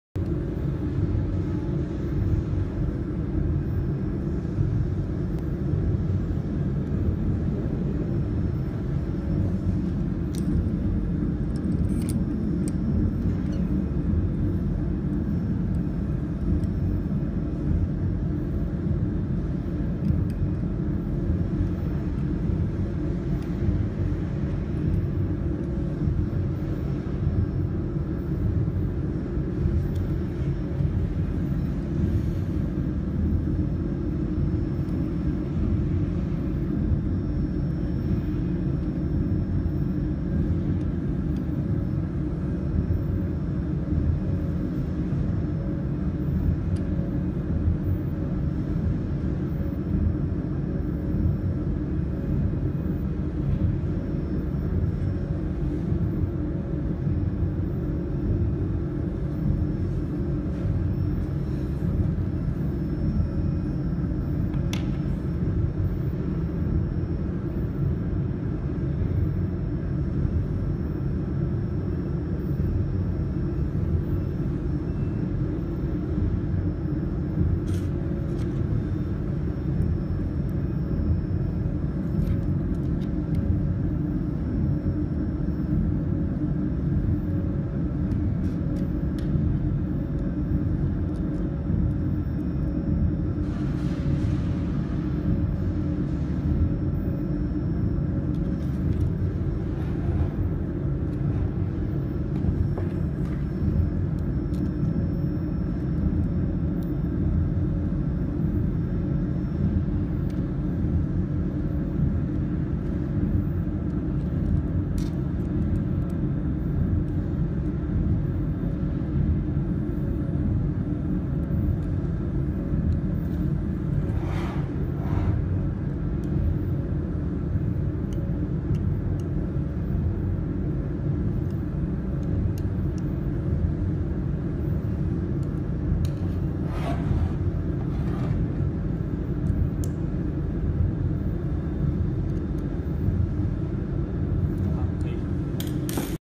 How to replace the xenon lamp
Sue
Shenzhen Tip-top International Manufacture&Trade Co. , Ltd

This is a  teaching video about how to replace the xenon lamp of the jewelry laser welding machine.